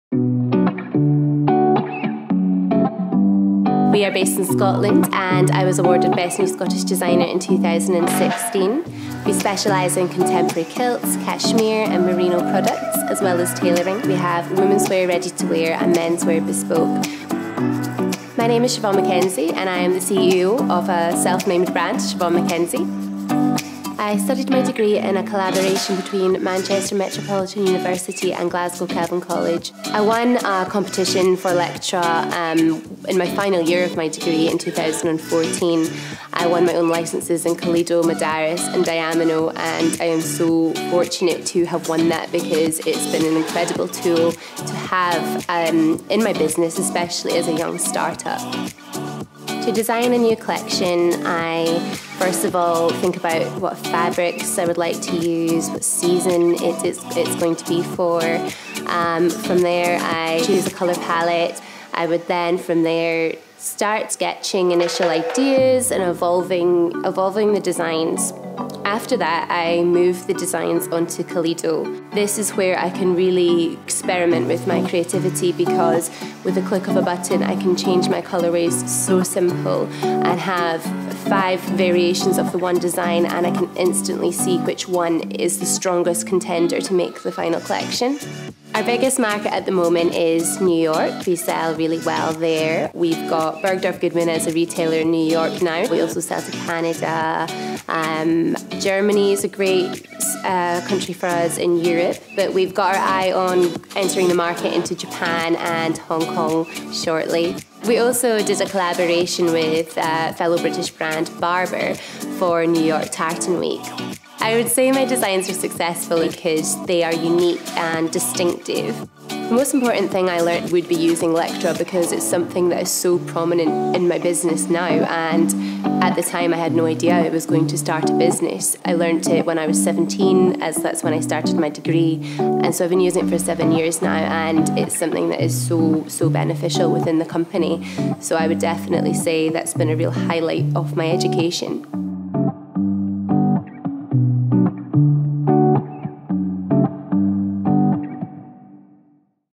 力克教育合作伙伴计划：Siobhan Mackenzie 访谈
Siobhan Mackenzie 毕业于曼彻斯特城市大学和格拉斯哥凯文学院。这位年轻的设计师在 2016 年获得“苏格兰最佳设计师”大奖，并且创立了以自己名字命名的男女苏格兰裙品牌。她介绍是如何使用力克的 Modaris 和 Diamino 软件（特别是 Kaledo）来设计自己的作品。